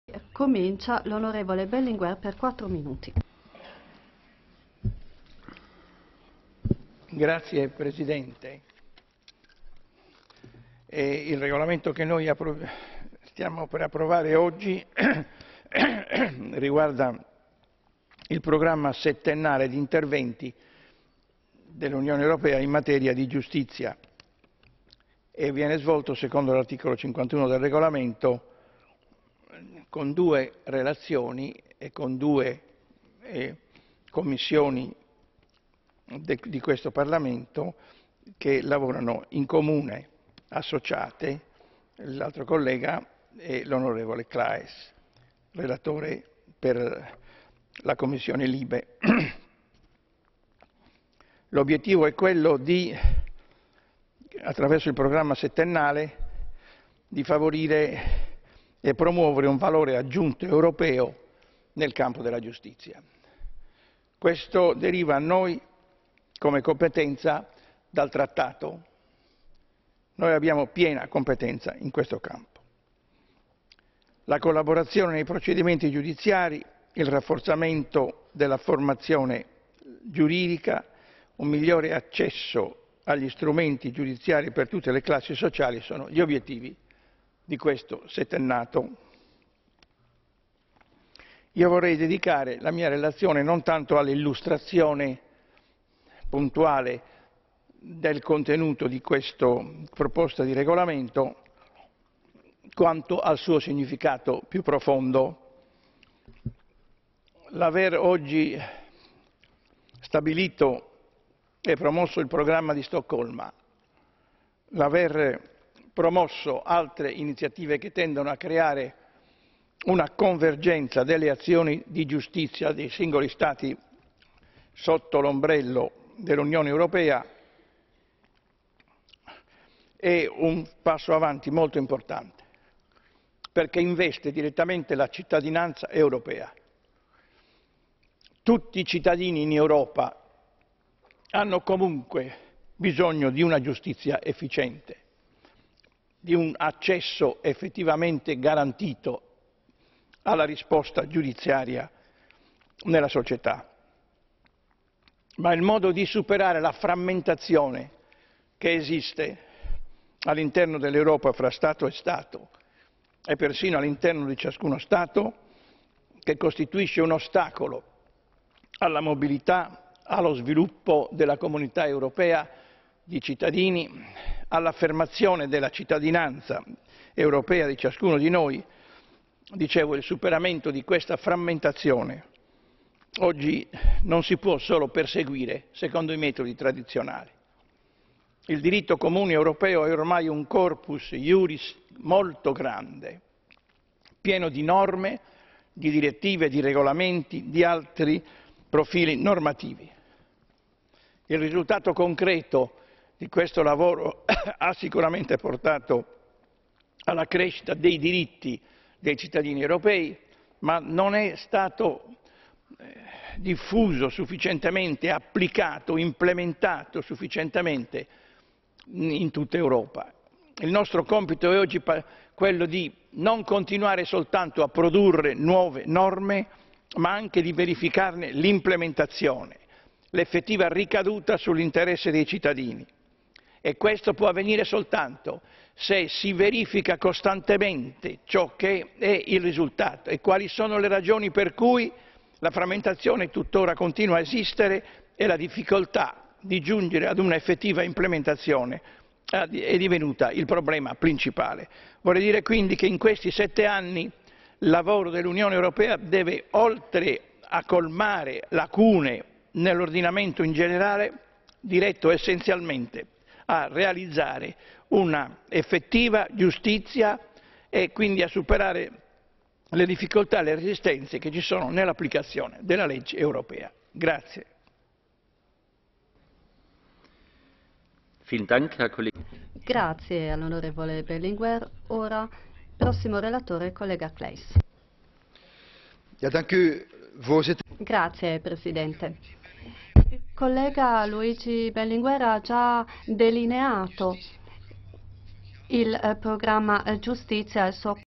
Berlinguer diritti e cittadini 10 12 13
Intervento in aula di Luigi Berlinguer sulla giustizia, i diritti e i cittadini